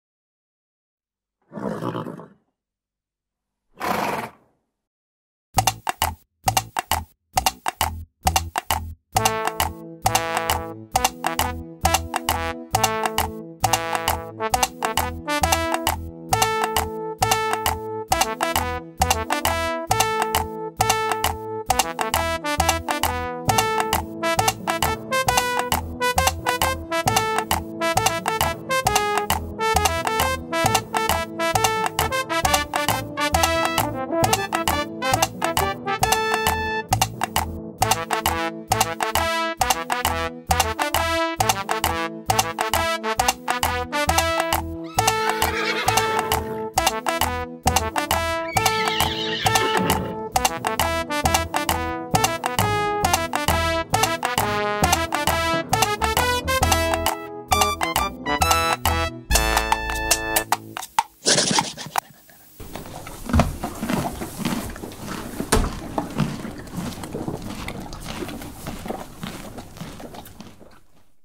Horse and Carrot Automaton
This is a video of an automaton I designed and made, featuring a galloping horse and a running carrot.
The full story is "When a cell of escaped lab rabbits released a GMO carrot, government scientists rushed to create a nanotech horse to restore ecological balance".
A detailed description of designing and making this appeared in the November/December 2022 edition of Automata Magazine, It's a great magazine, and well worth subscribing to, if you're interested in the amazing world of automata.
An explanation of the mechanism is also in the videos I made of the prototype horse and carrot, both of which are available from my YouTube channel.
The complete set of escaped lab rabbits, who are working frantically in the subterranean space to create more GMO carrots, will appear in a future YouTube Video.
BTW, I wrote and recorded the galloping music. It's been a dreadful ear worm, so apologies for that!
The horse sound effects are from the BBC sound library
Loads more info on my automata at grahamewaghorn.com